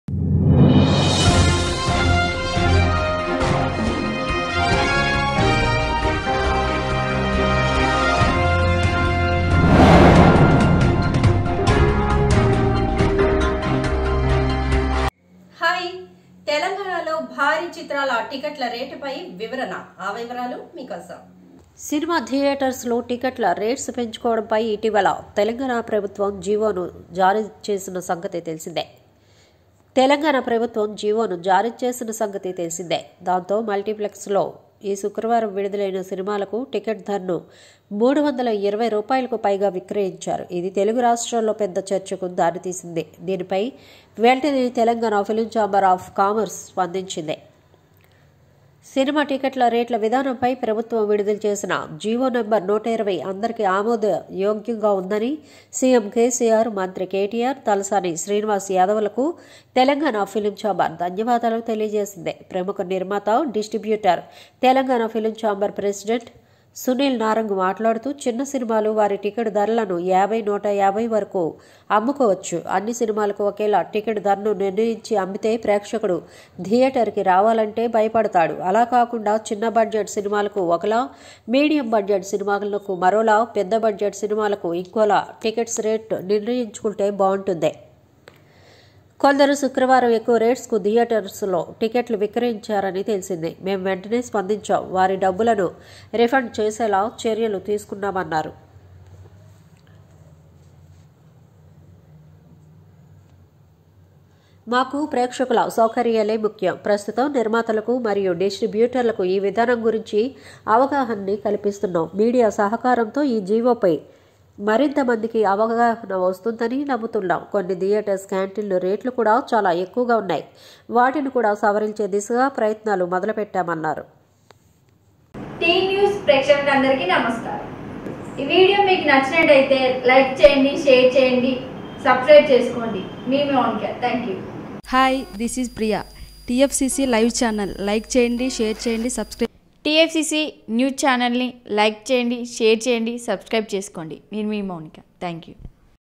RRR బడ్జెట్ గుట్టు చెప్పిన సునీల్ |తెలంగాణలో భారీ చిత్రాల టికెట్ రేట్స్‌పై క్లారిటీ ! TFCC NEWS
RRR బడ్జెట్ గుట్టు చెప్పిన సునీల్ |తెలంగాణలో భారీ చిత్రాల టికెట్ రేట్స్‌పై క్లారిటీ !